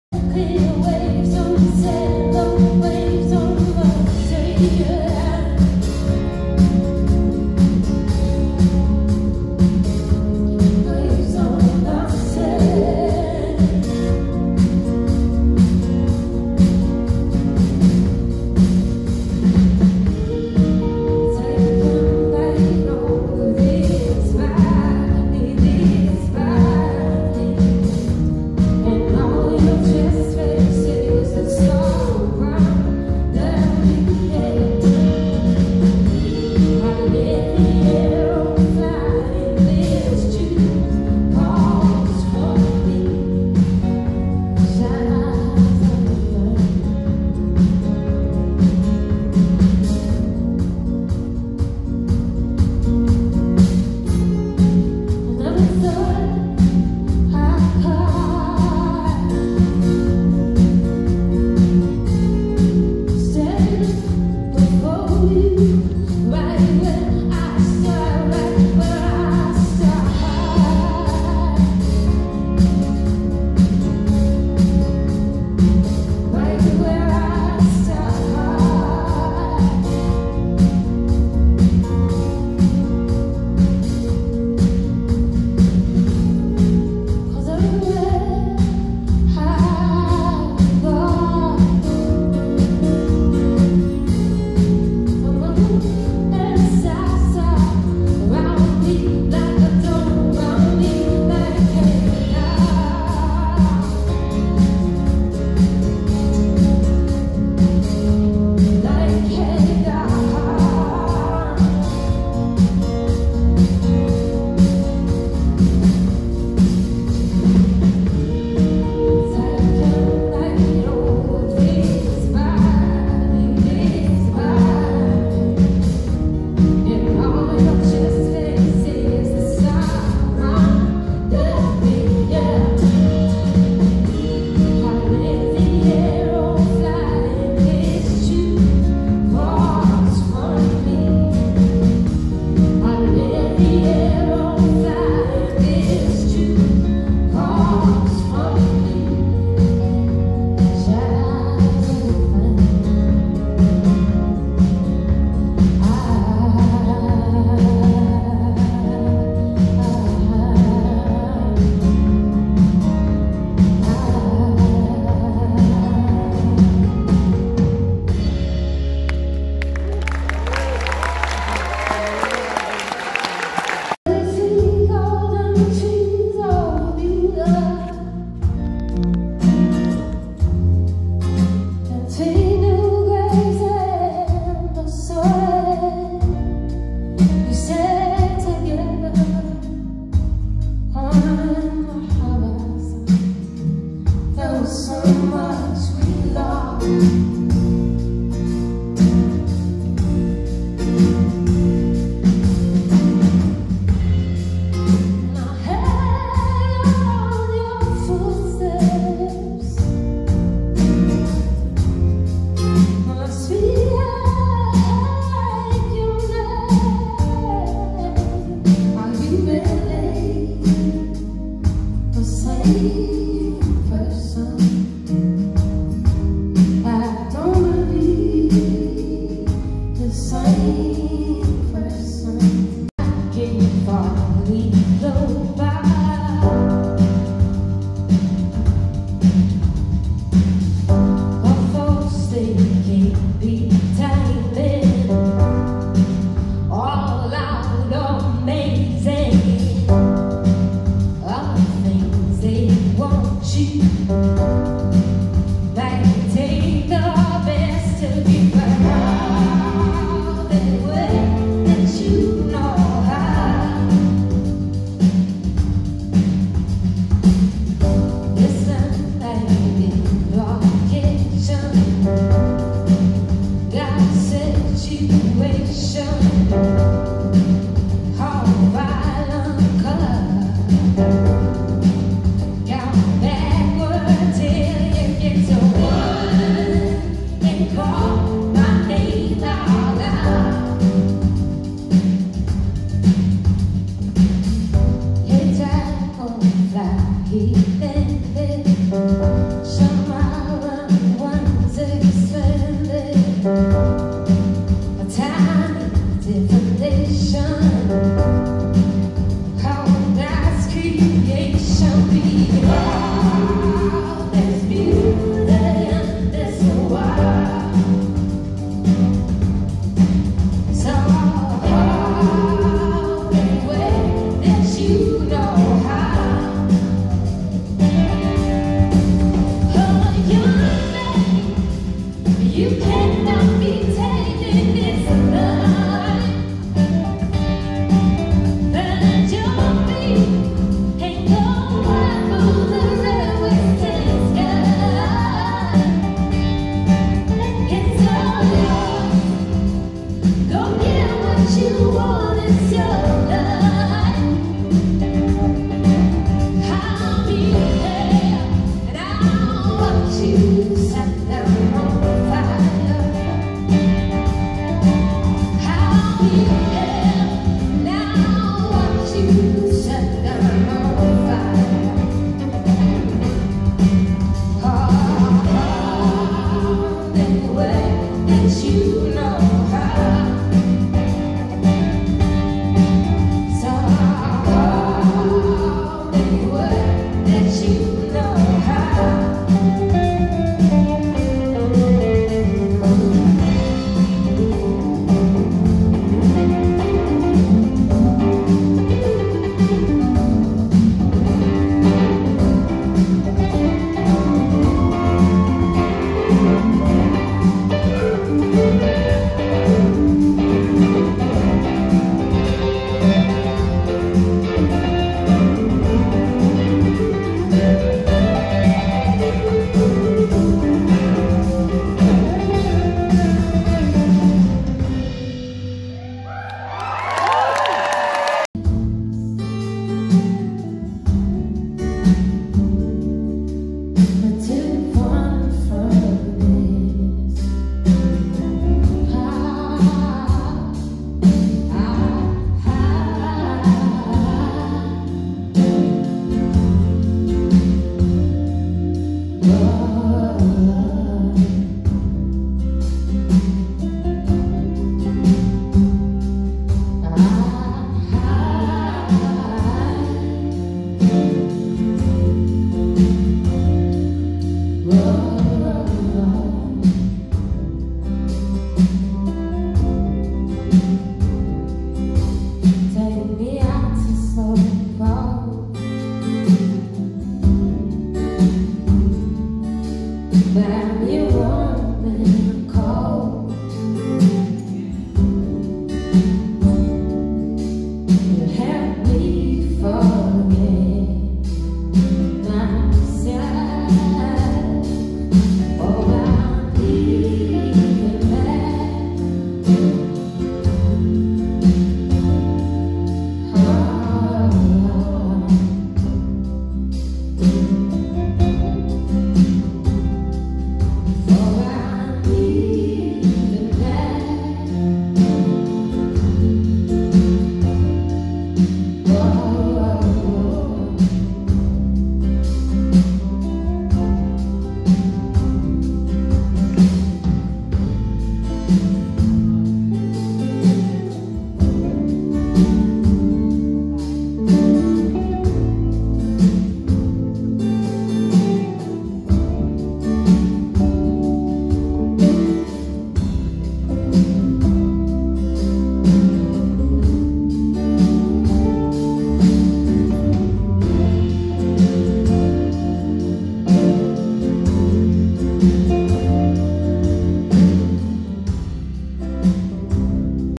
Frazey Ford at Port Theatre May 25th 2022.
Went with my mom, was my forth time seeing her. love that she makes her own clothes for her tours!!